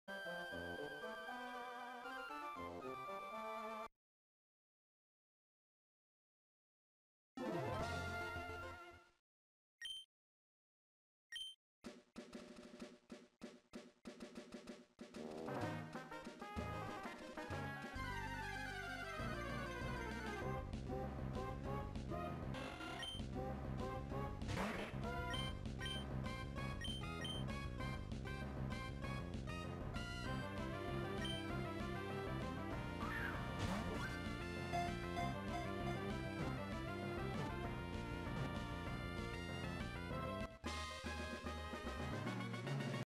wingull catch manip example
the second possible note you can enter the bag, then the third note of the melody change